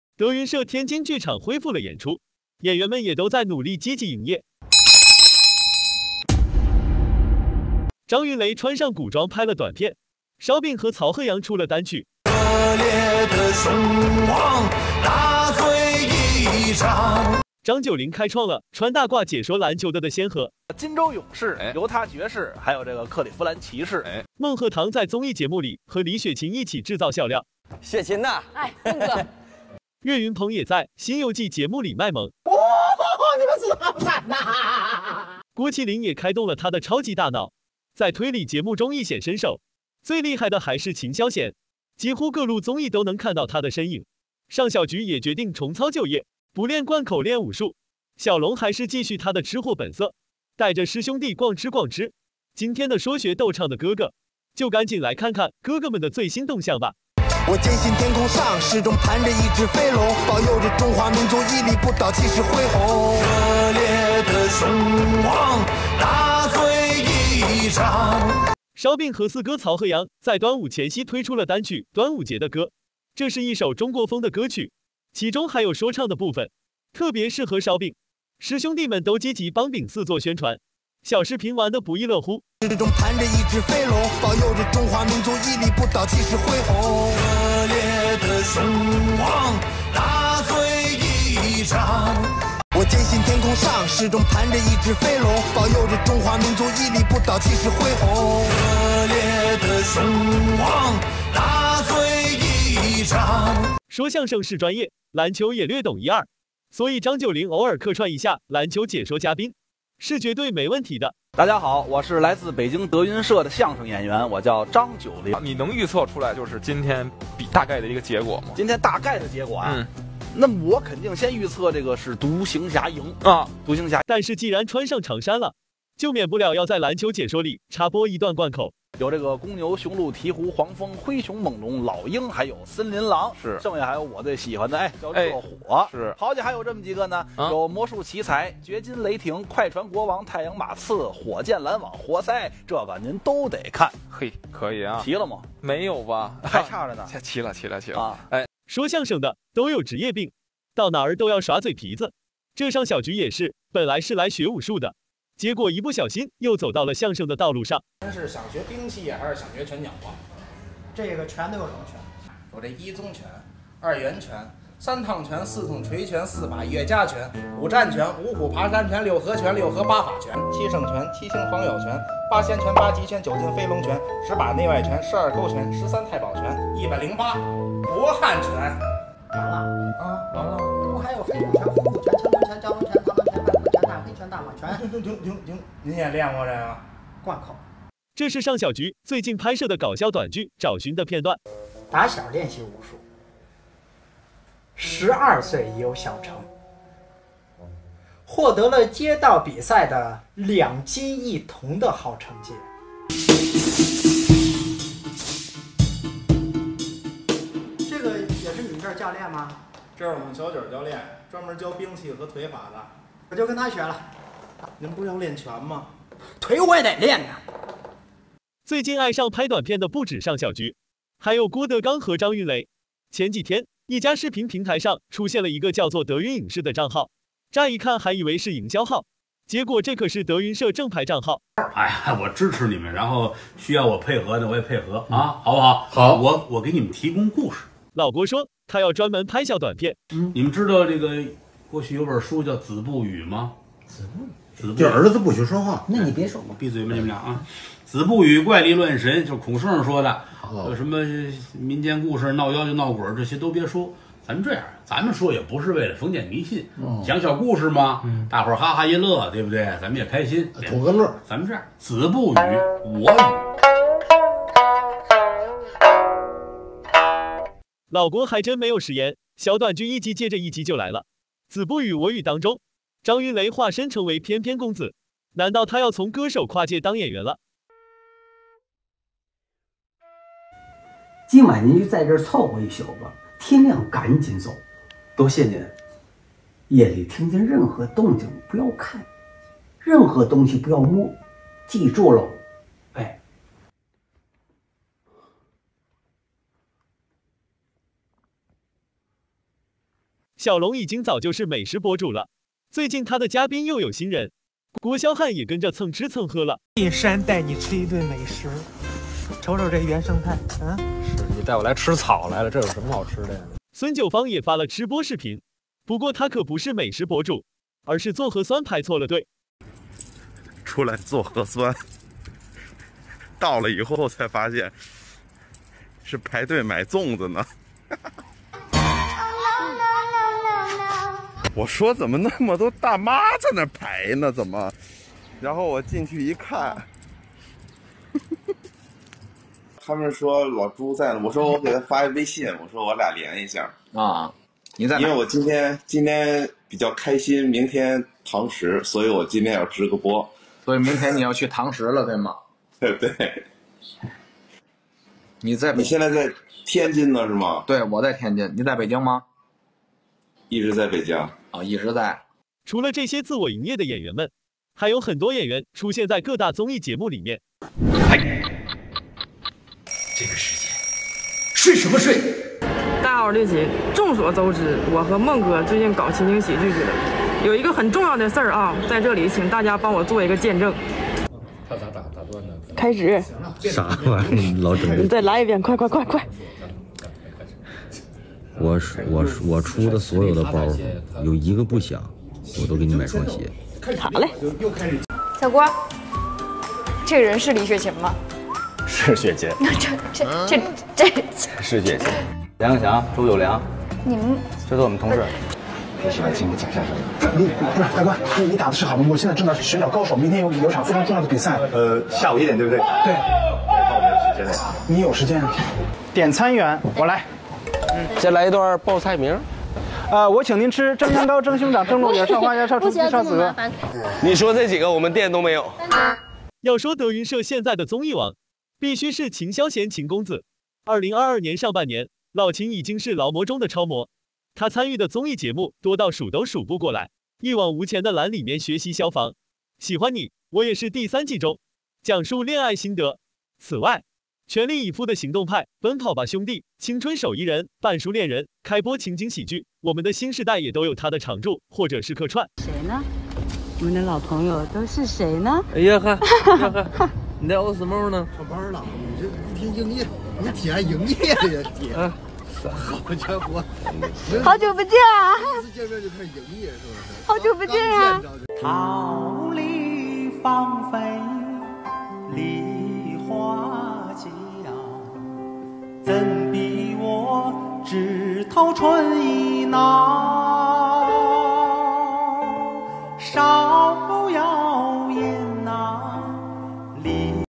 德云社最新动向：张云雷拍短剧，张九龄说篮球，饼四出新歌，尚筱菊练武术，郭麒麟、孟鹤堂、岳云鹏、秦霄贤录综艺火力全开
德云社天津剧场恢复了演出，演员们也都在努力积极营业：张云雷穿上古装拍了短片，烧饼和曹鹤阳出了单曲，张九龄开创了穿大褂解说篮球的的先河。孟鹤堂在综艺节目里和李雪琴一起制造笑料，岳云鹏也在《新游记》节目里卖萌，郭麒麟也开动了他的超级大脑，在推理节目中一显身手，最厉害的还是秦霄贤，几乎各路综艺都能看到他的身影。尚筱菊也决定重操旧业，不练贯口练武术。小龙还是继续他的吃货本色，带着师兄弟逛吃逛吃。今天的《说学逗唱的哥哥》，就赶紧来看看哥哥们的最新动向吧。